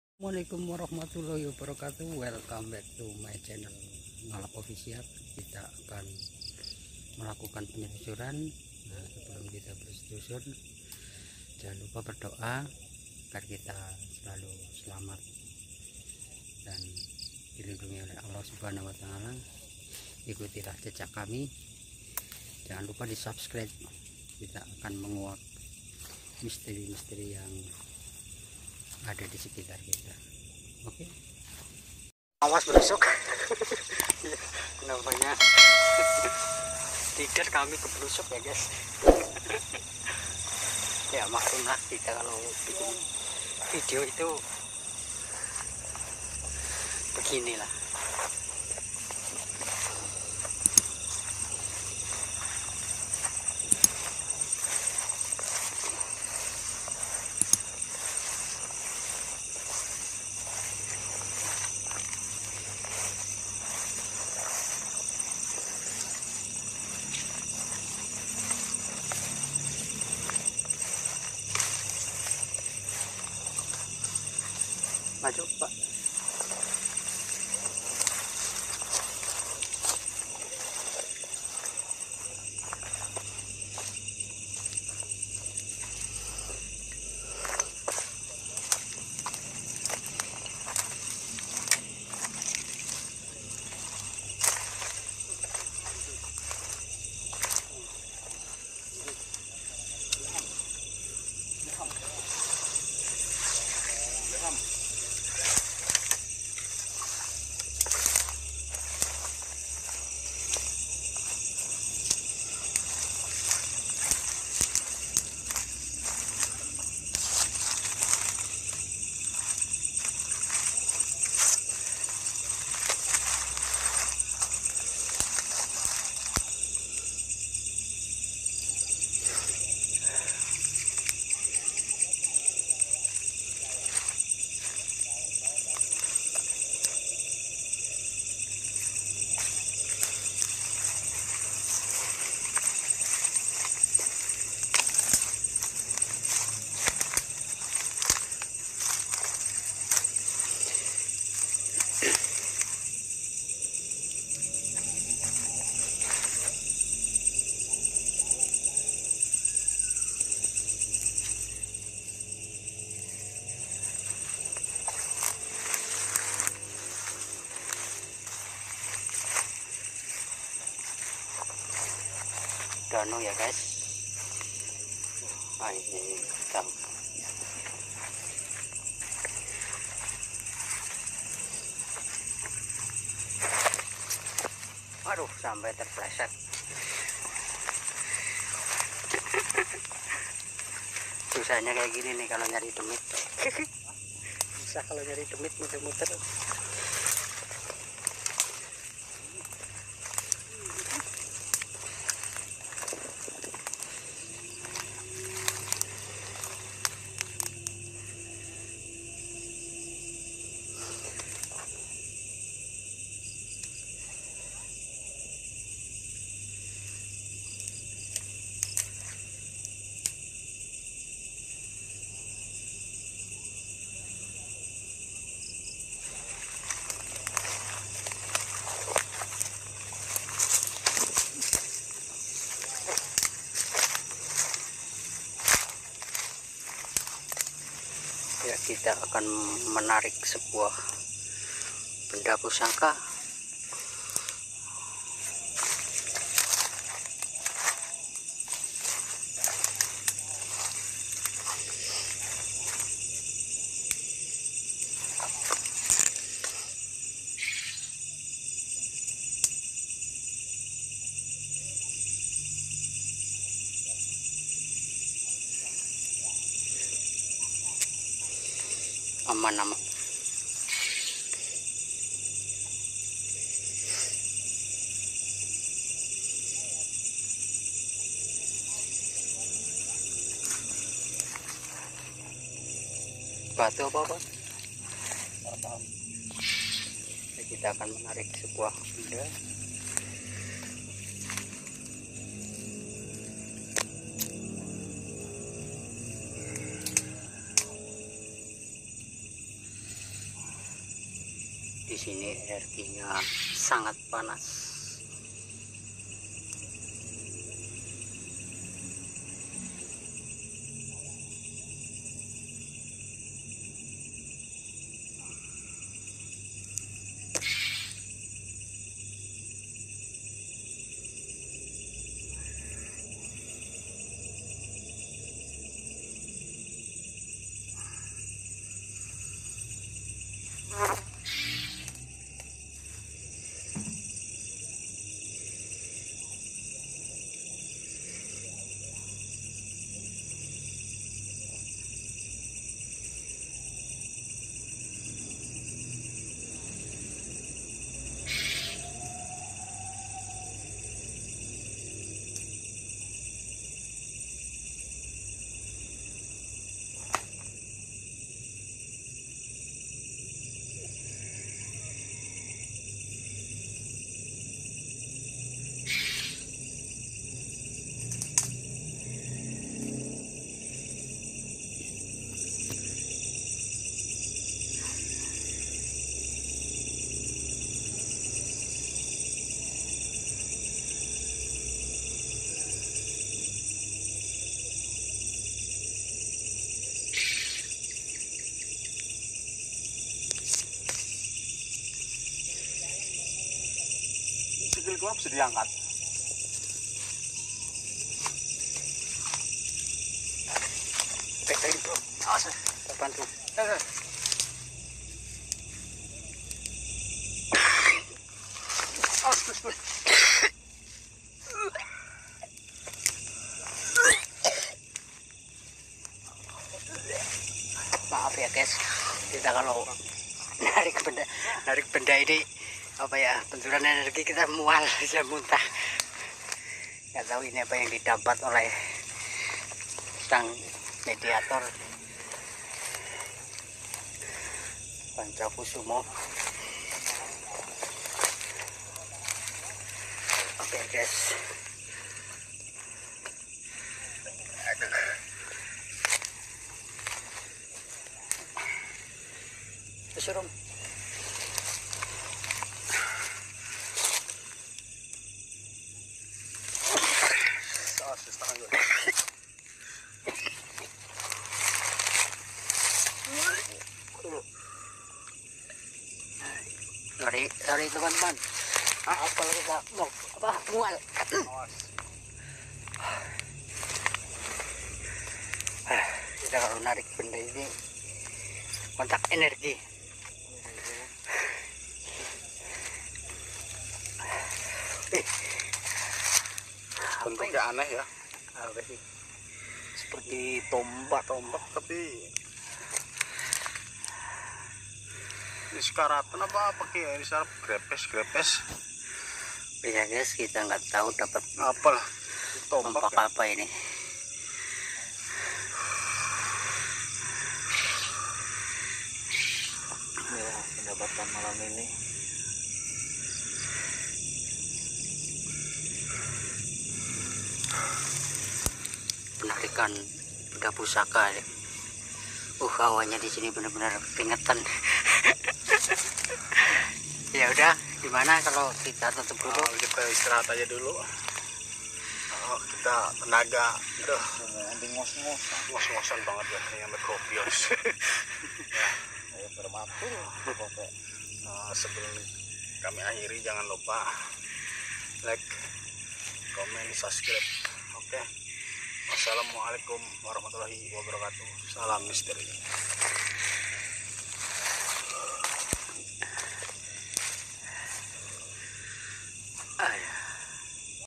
Pusaka Tombak Angrem Misterius | penelusuran tempat angker
kami tidak memaksakan apa yang kami percaya untuk kalian percaya.kami hanya ingin berbagi cerita.
selamat menonton video ini di buat bertujuan hanyalah hiburan terima kasih.